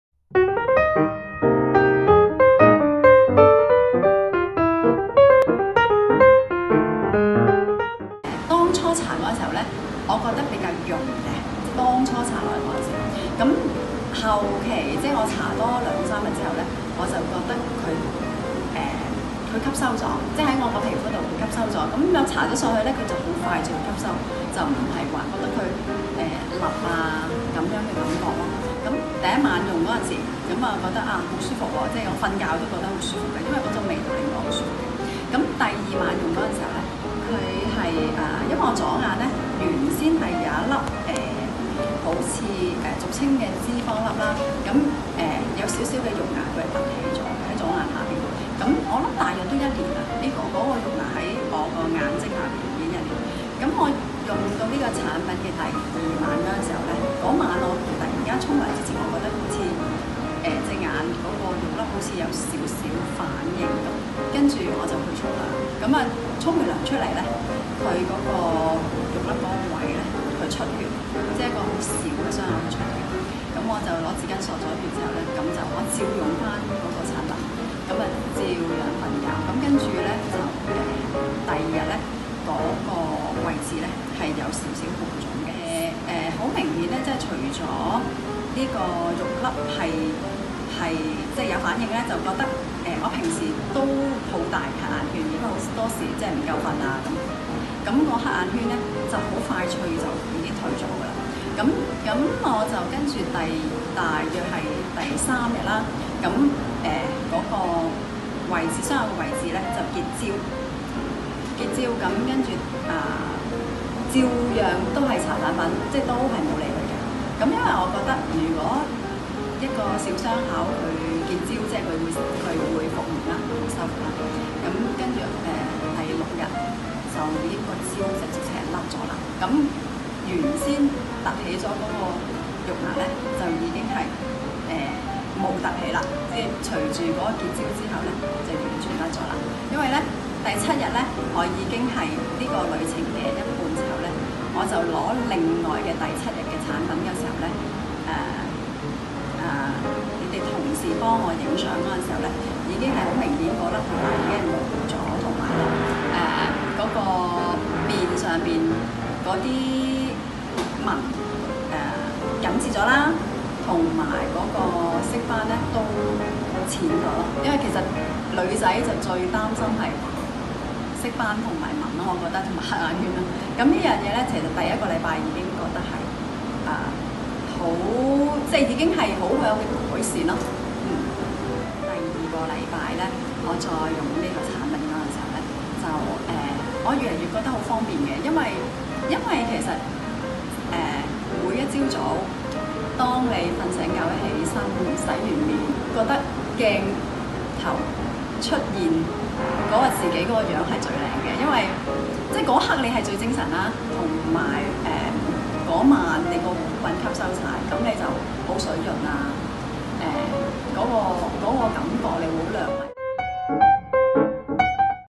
【用家見證】不丹喜來芝14天返齡之旅～何小姐：困擾的油脂粒掉下來了！｜綠色東方
不丹喜來芝 14天返齡之旅 第二名🏅得獎者係💃🏻舞蹈老師何小姐，佢嘅旅程體驗最深刻就係《甩掉困擾的油脂粒》！何老師係活動當日話俾粒油脂粒困擾了一年，用咗好多方法都甩唔到，估唔到用咗幾日喜來芝就自己甩咗！好驚喜～ 👏🏻

另外，何老師非常認真試用產品，大家亦都見到佢前後對比好明顯臉部比之前緊緻、亮澤，細紋同色斑都減淡咗，真係實至名歸呀。恭喜晒～